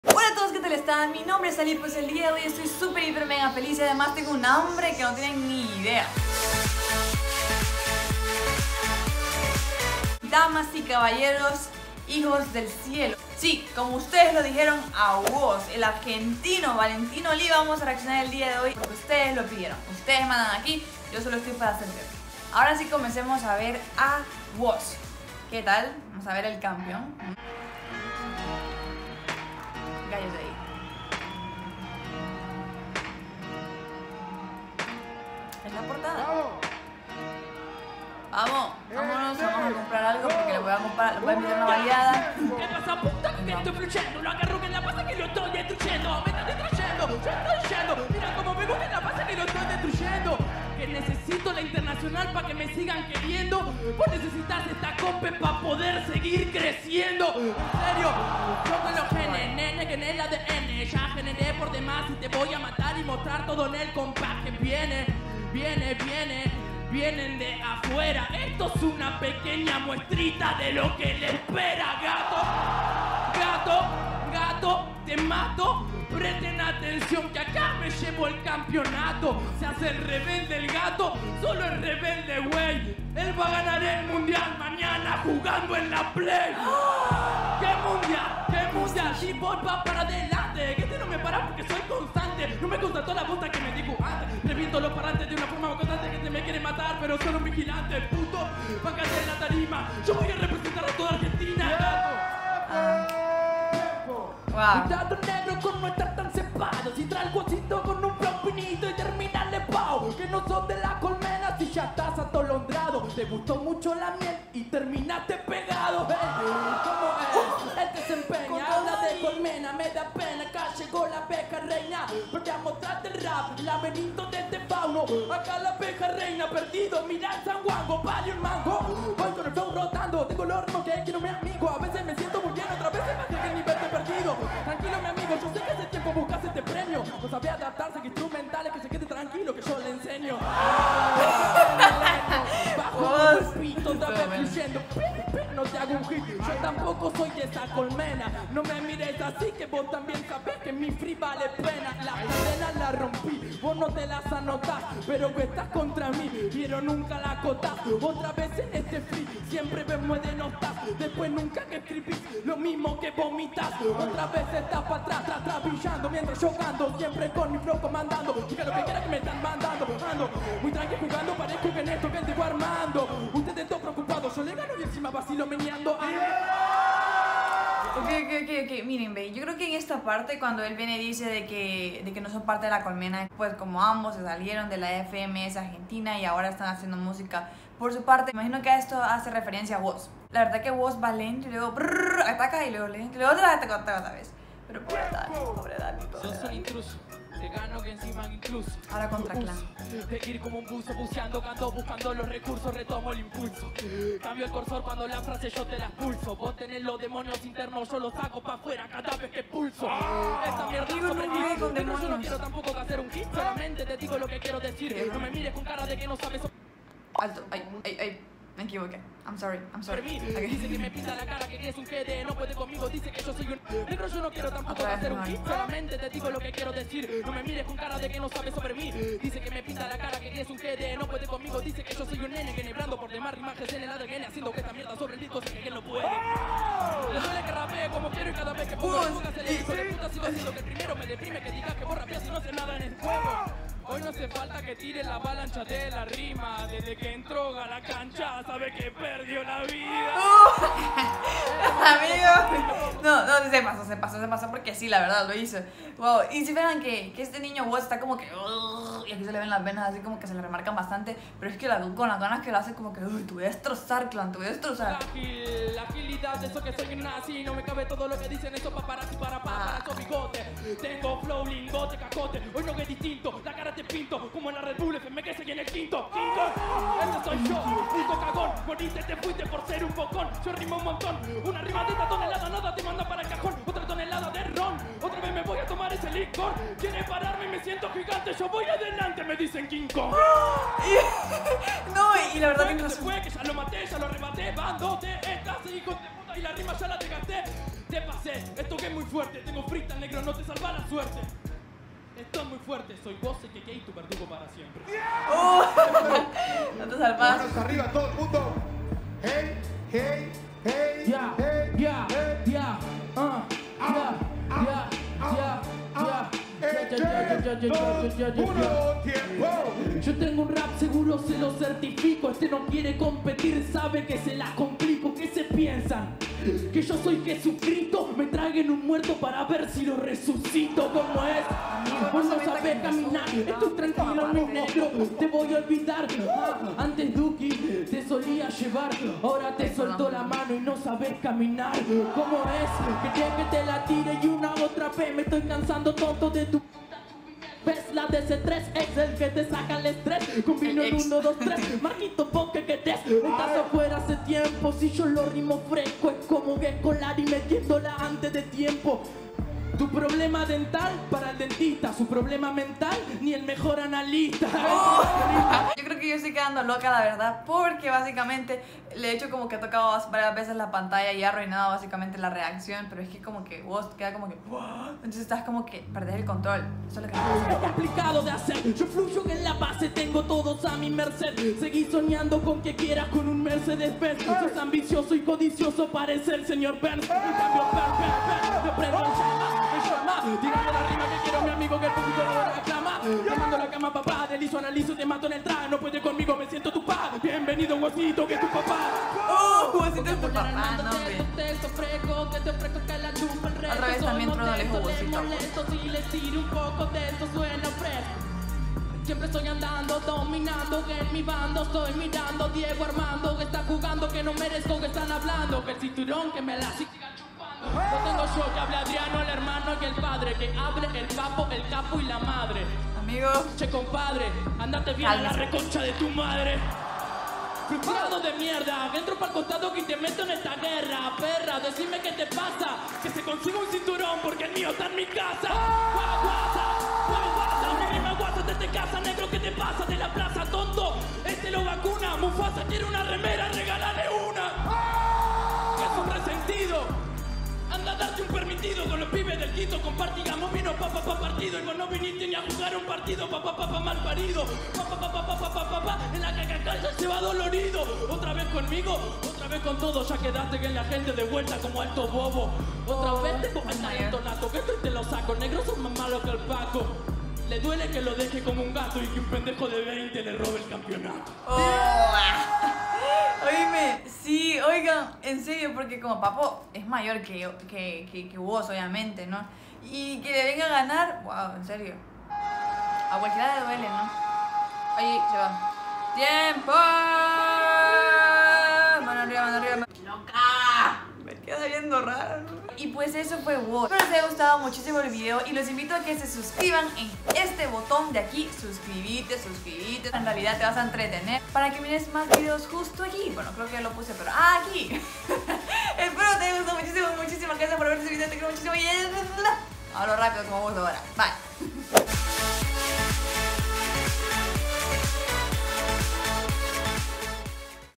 REACCIÓN a lo MEJOR de WOS !!! { 0000} 🚹 FMS ARGENTINA
🚹La REACCIÓN más LOCA a lo MEJOR a WOS EL CAMPEÓN 🚹Tenes QUE VERLO ⚡️,te vas a REÍR un RATO

Hola que tal estas! Soy Ally

HABLEMOS un RATO ❤️

Viajes
Cortometrajes
Baile
Tutoriales
Reacciones
Consejos

Me gusta hacer de todo